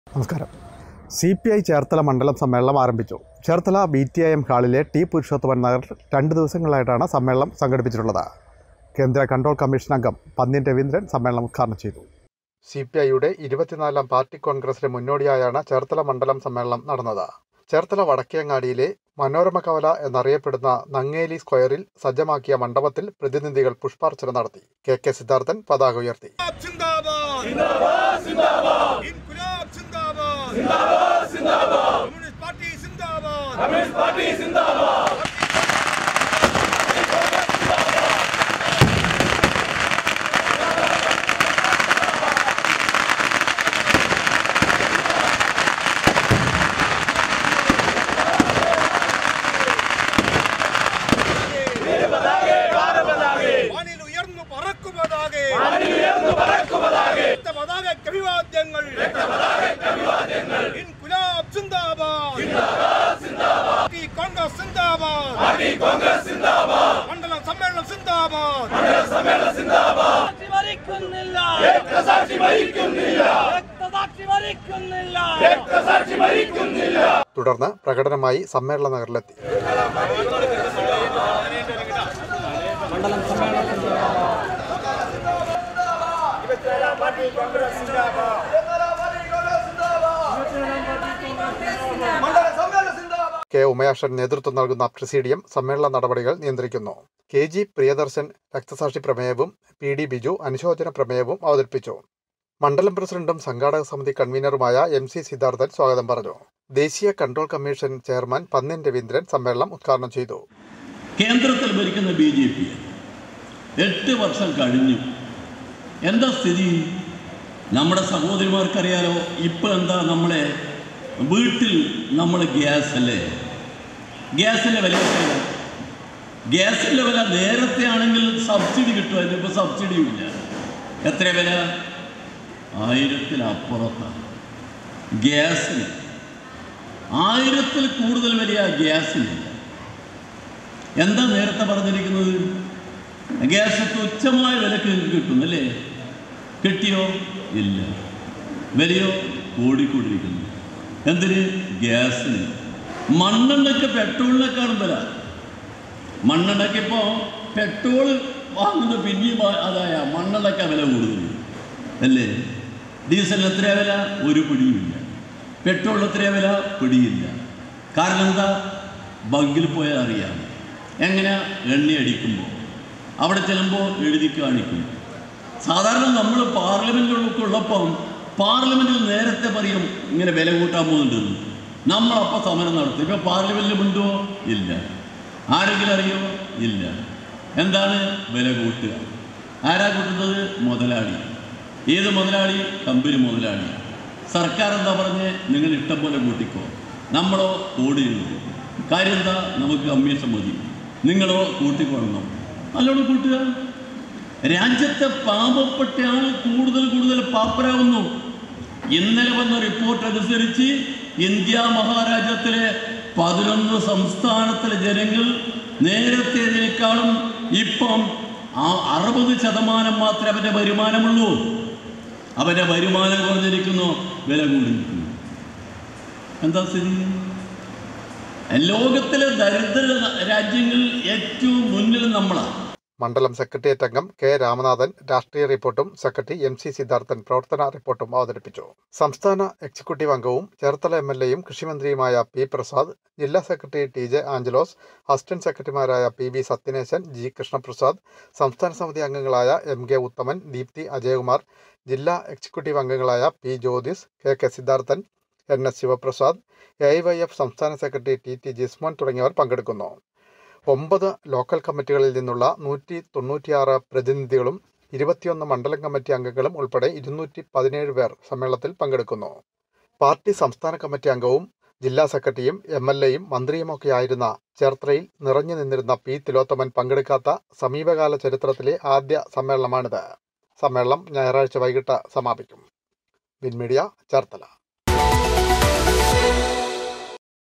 സി.പി.ഐ. ചേർത്തല മണ്ഡലം സമ്മേളനം ആരംഭിച്ചു.
സി.പി.ഐ. ചേർത്തല മണ്ഡലം സമ്മേളനം ആരംഭിച്ചു. ചേർത്തല VTAM ഹാളിൽ രണ്ട് ദിവസങ്ങളിലായാണ് സമ്മേളനം സംഘടിപ്പിച്ചിട്ടുള്ളത്.